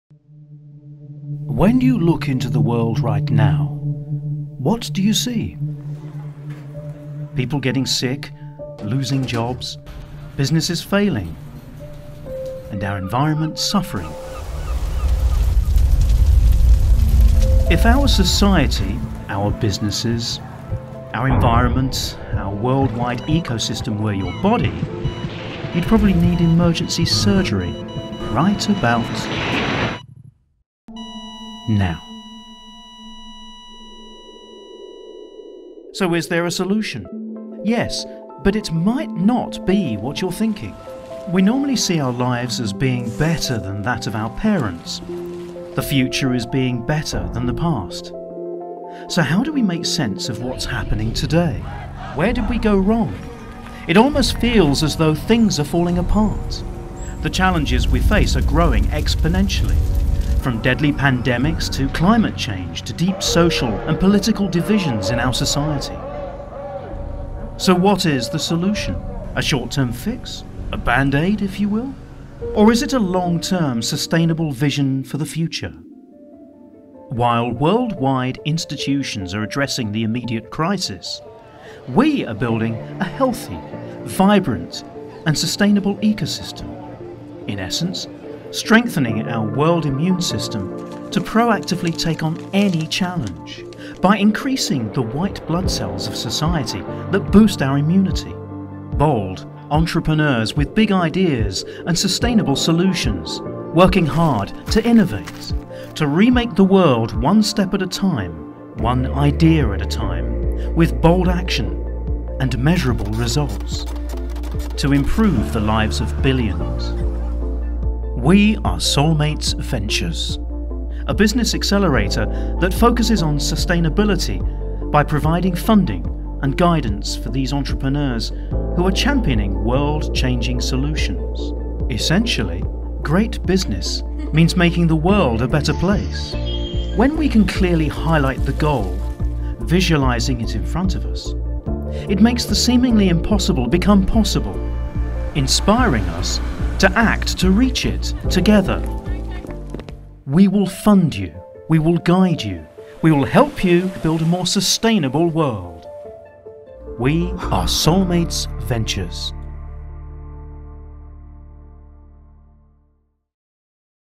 Soulmates Ventures – Brand Film Launch
Soulmates Ventures is a green investing accelerator designed for your growth. With a strong focus on social impact and environmental sustainability, we help accelerate companies that sit at the exciting intersection between world-class technology and innovative solutions. Jump in!

We would love your help sharing our story with startups, investors, colleagues, friends, and family!

Thank you to the Soulmates team that shared their time and talent to help us tell our story.

CREDITS:
Scripted, directed and edited by Adad Joel Warda